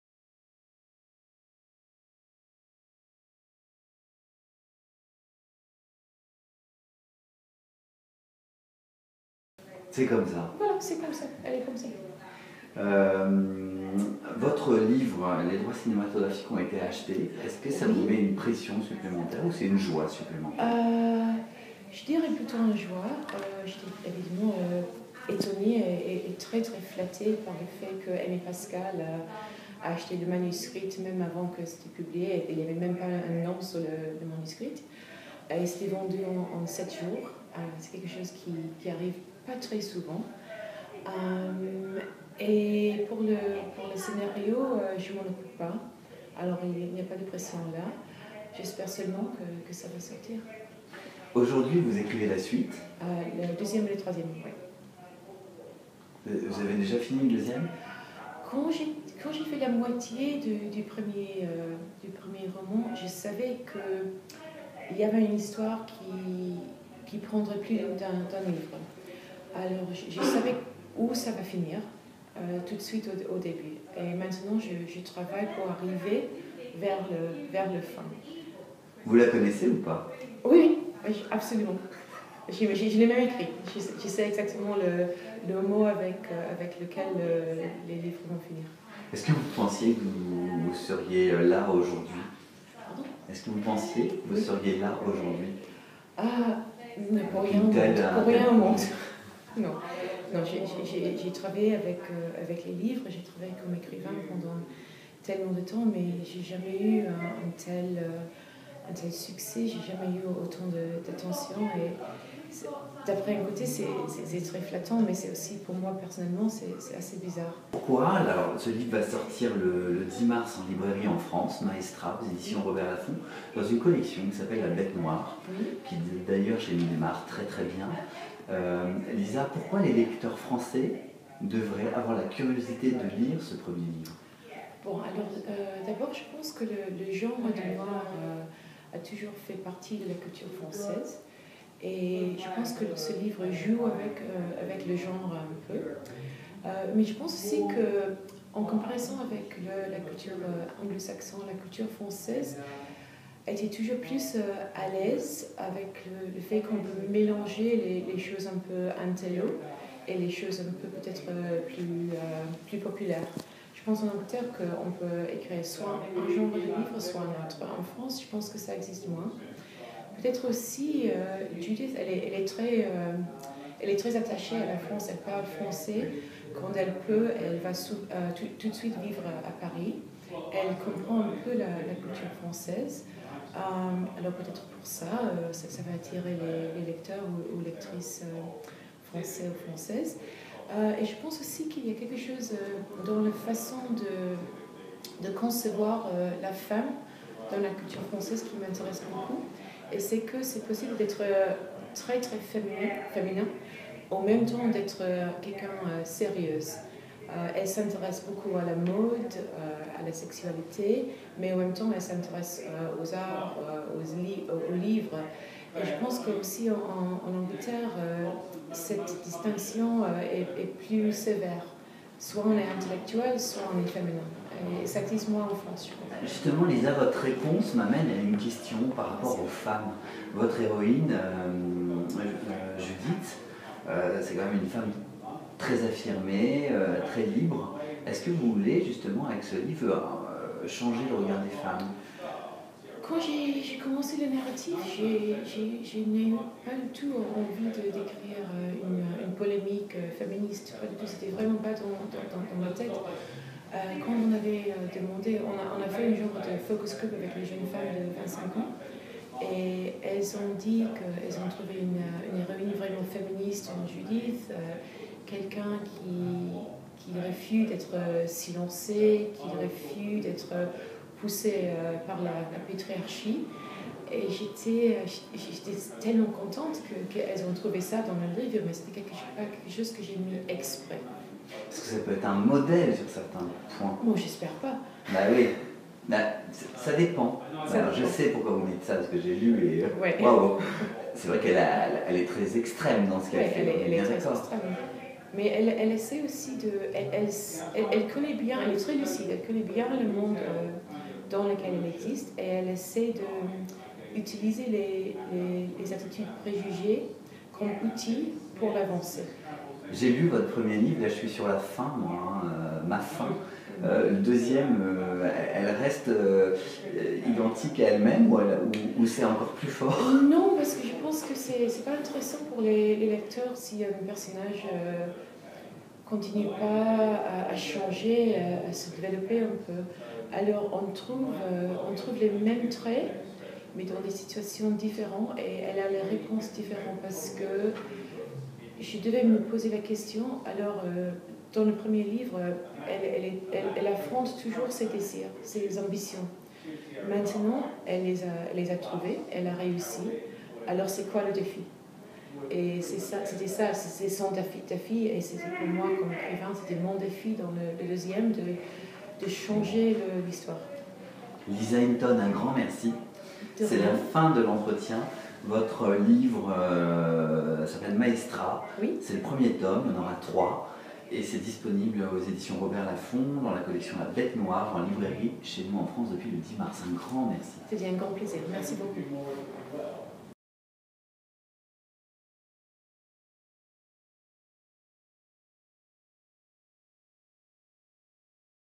Lisa Hilton : "Ce livre joue avec plusieurs genres"
Deuxième partie de notre rencontre à Londres avec Lisa Hilton pour la sortie de son livre "MAESTRA" aux Éditions Robert Laffont / La bête noire.